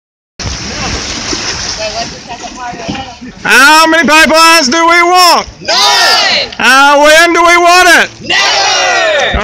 How many pipelines do we want? None! When do we want it? @ WWALS 2015-08-15
Sabal Trail nope! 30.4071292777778, -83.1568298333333
Alapaha Rise, Sabal Trail, Five Holes, Suwannee River, Withlacoochee Confluence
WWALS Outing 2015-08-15
Videos by John S. Quarterman for WWALS Watershed Coalition, Inc.